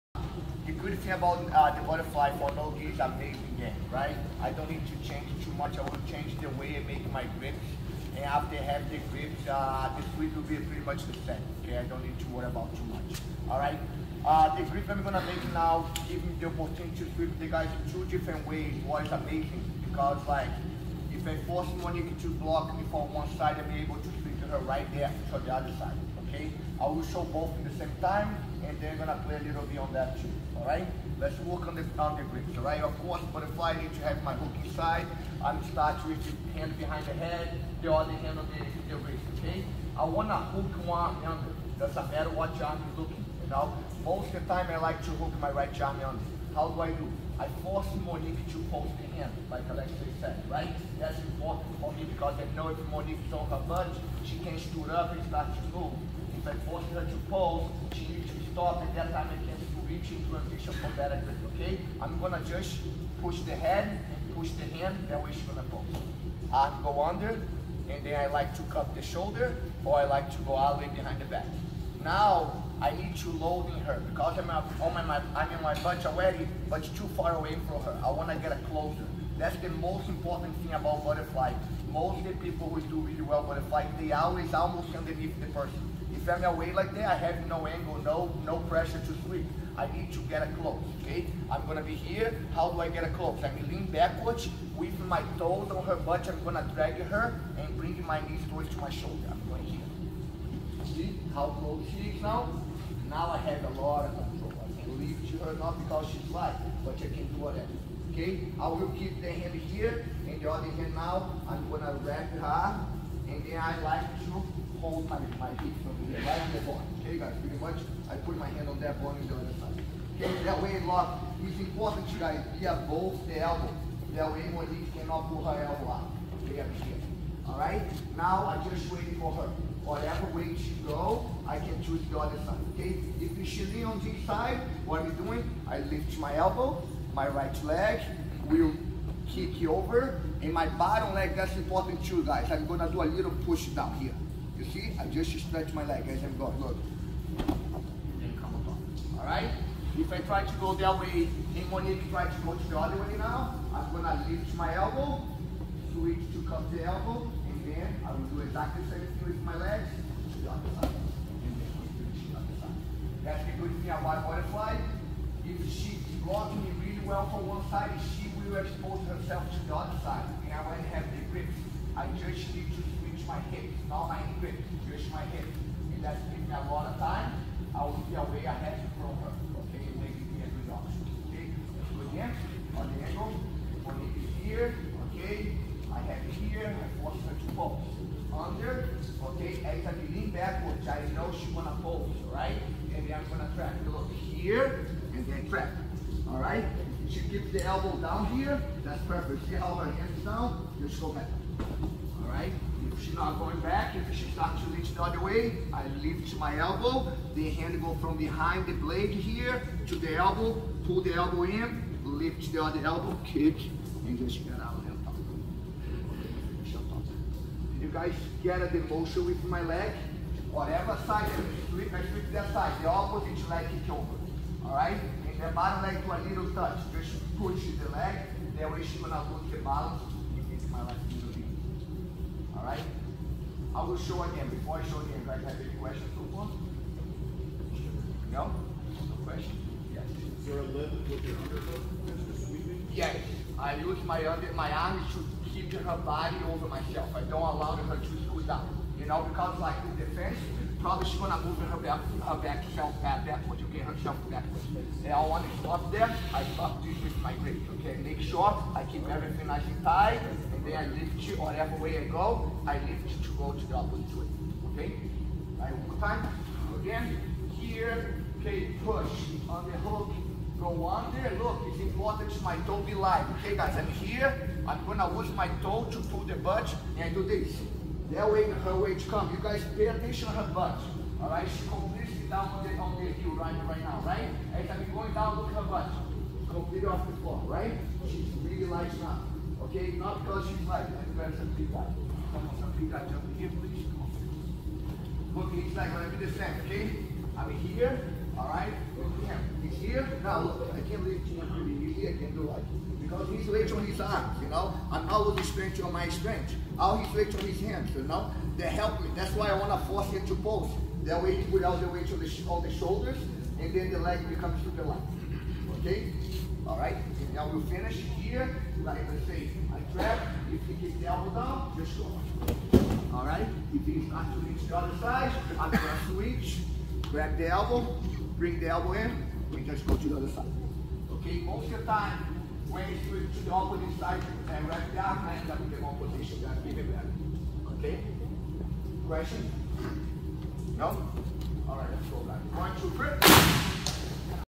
Rafael Formiga: NoGi Butterfly Guard Sweep
Rafael Formiga shares details on the butterfly guard sweep at Origin Immersion Camp 2022.